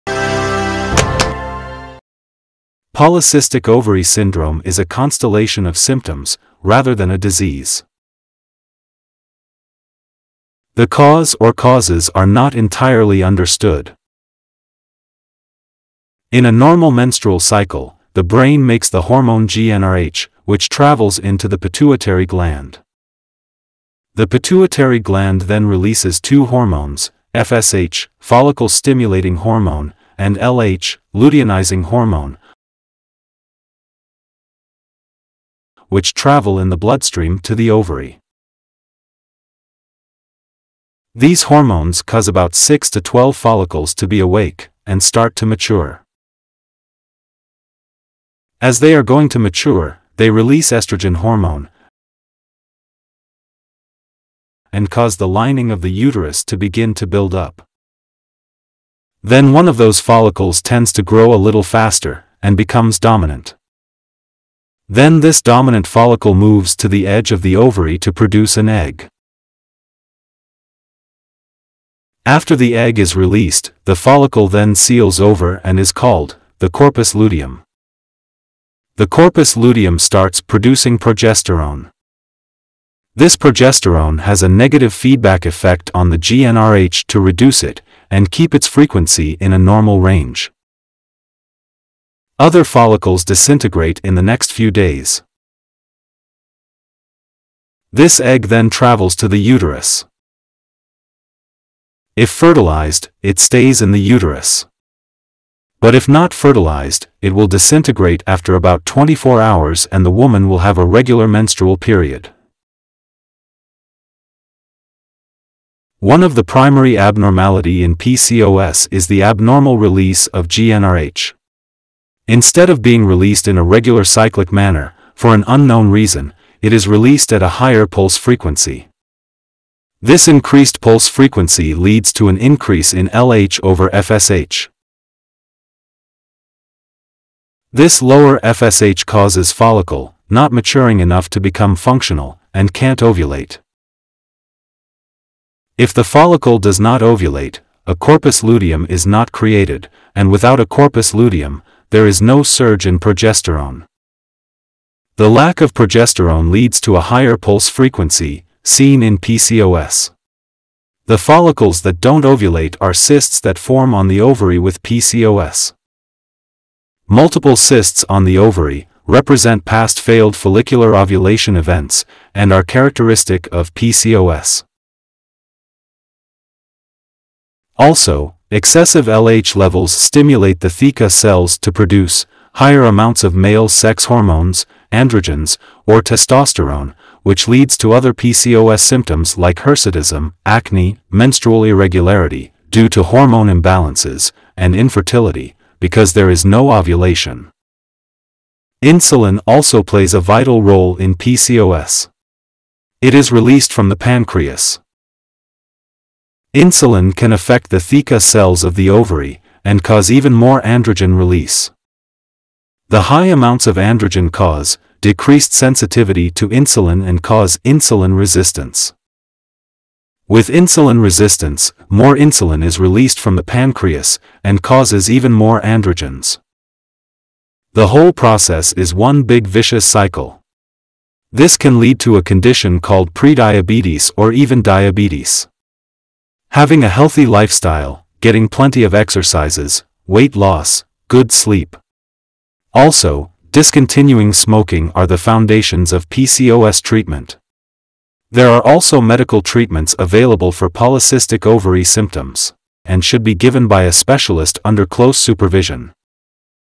Menstruations and Polycystic ovary syndrome PCOS ( 3D Animation )
Menstruation is a woman's monthly normal vaginal bleeding, often called the period.
Polycystic Ovary Syndrome (PCOS), is  a common hormonal disorder affecting millions of women worldwide.
Polycystic ovary syndrome is a constellation of symptoms, rather than a disease. The cause or causes are not entirely understood.

What You'll Learn:
What is PCOS?

Definition and overview of PCOS.
Prevalence and impact on women's health.
How PCOS Happens:

The role of hormones: Androgens, Insulin, and LH.
Genetic and environmental factors.
The development of cysts in the ovaries.
Symptoms and Diagnosis:

Common symptoms: Irregular periods, weight gain, acne, hair growth.
Diagnostic criteria: Ultrasound, blood tests, and physical exams.
Treatments for PCOS:

Lifestyle changes: Diet, exercise, and weight management.
Medications: Birth control pills, anti-androgens, and insulin-sensitizing drugs.
Fertility treatments: Clomiphene, Metformin, and In Vitro Fertilization (IVF).
Alternative therapies: Herbal remedies, acupuncture, and stress management.
Living with PCOS:

Tips for managing symptoms.
Mental health considerations.
Support networks and resources.
Whether you're newly diagnosed, know someone with PCOS, or are simply curious about this condition, this video provides a comprehensive understanding of PCOS and the various treatment options available.

HealthEducation Medical Awareness.